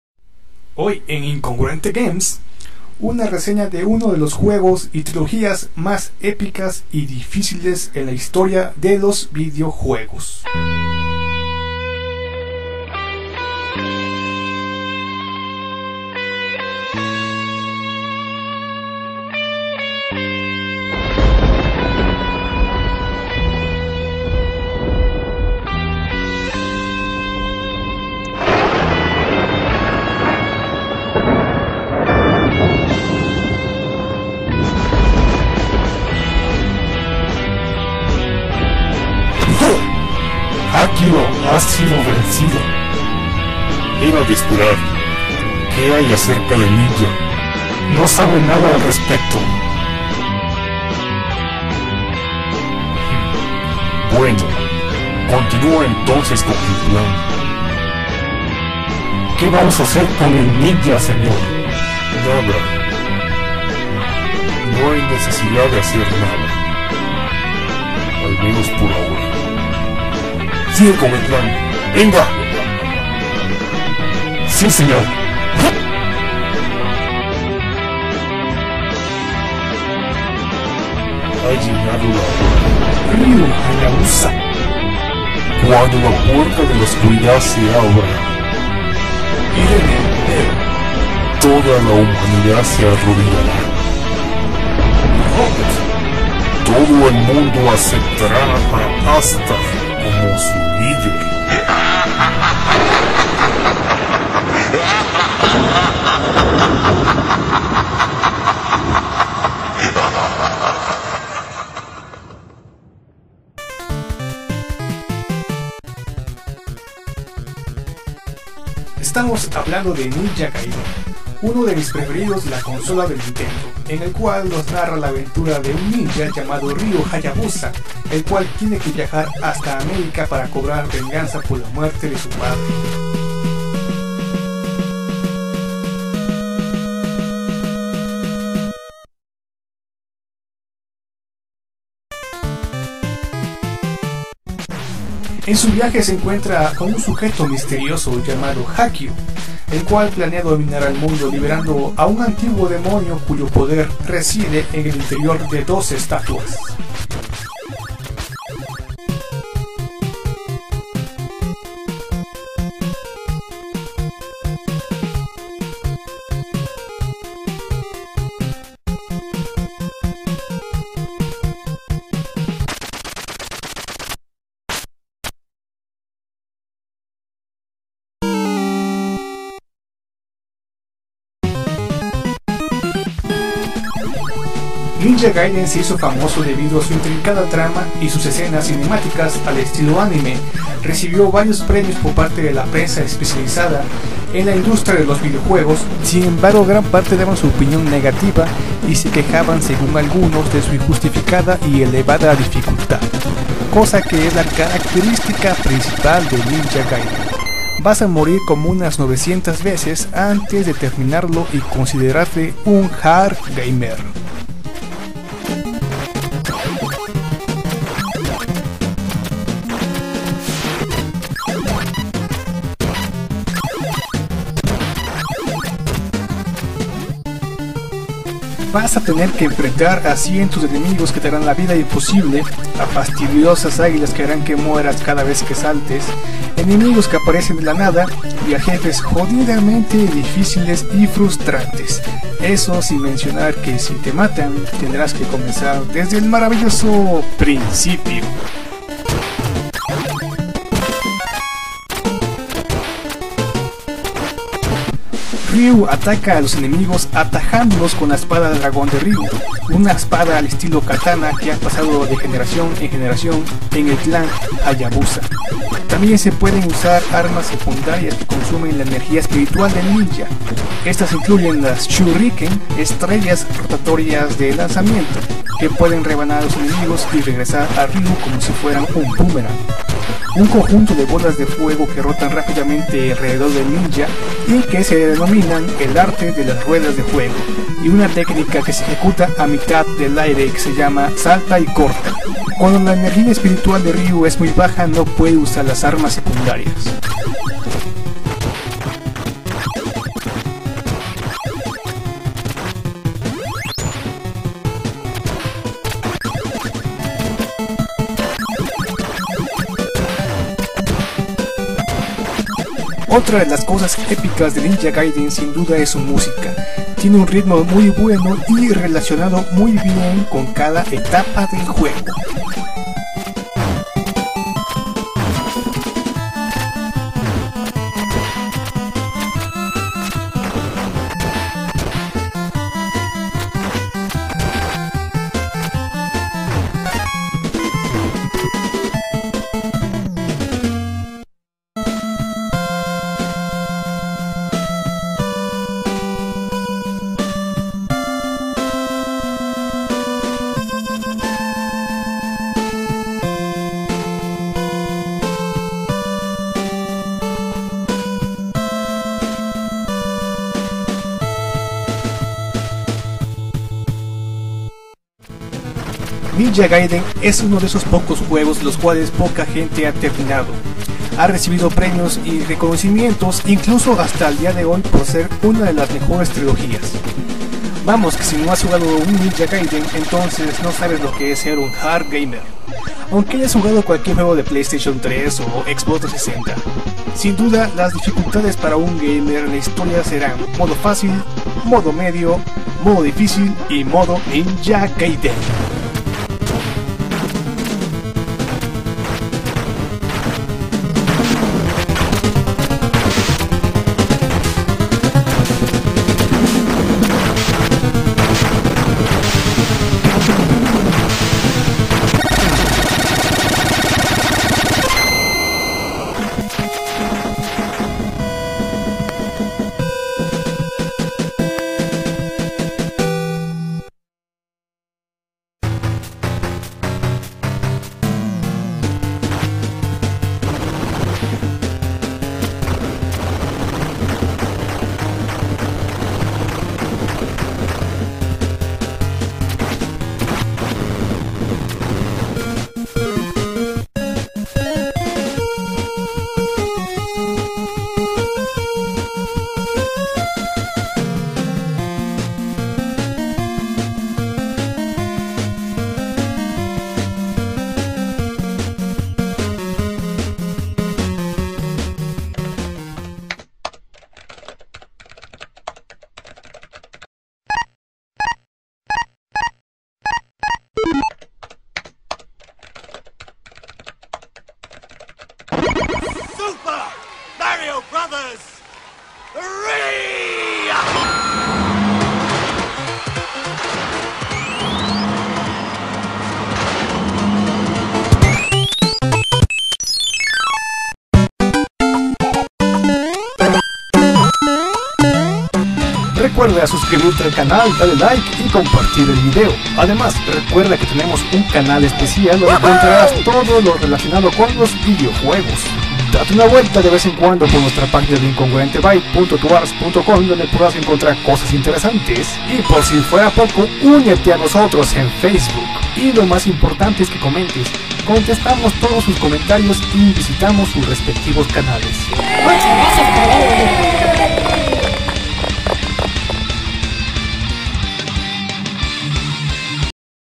Ninja Gaiden Nes - Incongruente Games [Reseña Español]
Si jugaste ya con este ninja en PS3 o XBOX 360, y no sabías o no recordabas que tenía un largo pasado dentro de ese misterioso disfraz, ahora te traigo aquí el recordatorio de la saga NINJA GAIDEN.

La historia de Ninja Gaiden sigue las aventuras del ninja del Clan del Dragón, Ryu Hayabusa. El estilo de juego es del tipo de acción de lado (comúnmente llamado "de plataforma")

en la cual es necesario ir pasando por varias etapas. Cada etapa consiste en un escenario el cual es necesario atravesar destruyendo o evitando a los enemigos en el camino. Tras unas cuantas etapas es posible enfrentarse a un "jefe". La finalización del escenario que enfrenta a un "jefe" concluye una parte del juego, que es llamada en forma dramática "Acto",

JackIncongruente // Paginas Principales \\